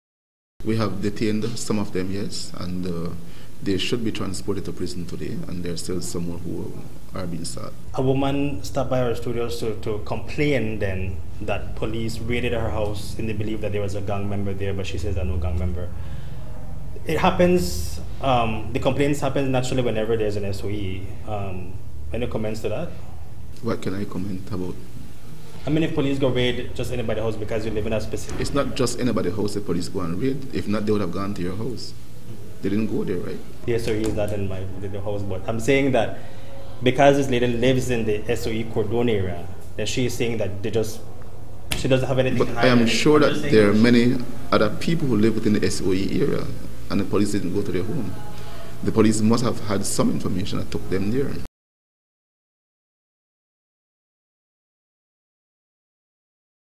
Amid Public Outcry, ComPol Says SOE Targeted Specific Individuals
A state of emergency was declared in three Belize City neighborhoods on Friday, bringing to a halt an unrelenting spate of gun violence that claimed as many as five lives.  During the predawn operation, specific individuals believed to be actively involved in the ongoing feud were rounded up and incarcerated for an initial period of thirty days.  Since then, the Belize Police Department has issued wanted posters for others who may have absconded.  Nonetheless, the families of several of these young men have turned to the media with complaints that the sweep at the start of the extended weekend was indiscriminate.  Commissioner of Police Chester Williams refutes those allegations.